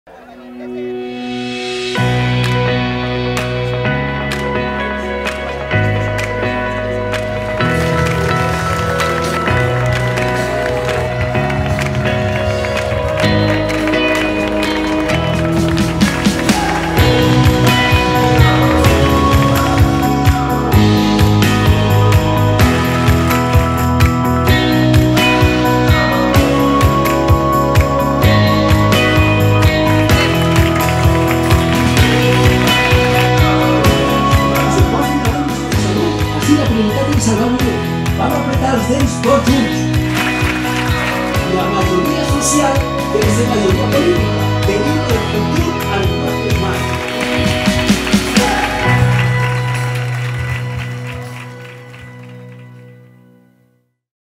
Resumen Vila-real
Vídeo resumen del acto realizado en Vila-real con la participación de Ximo Puig.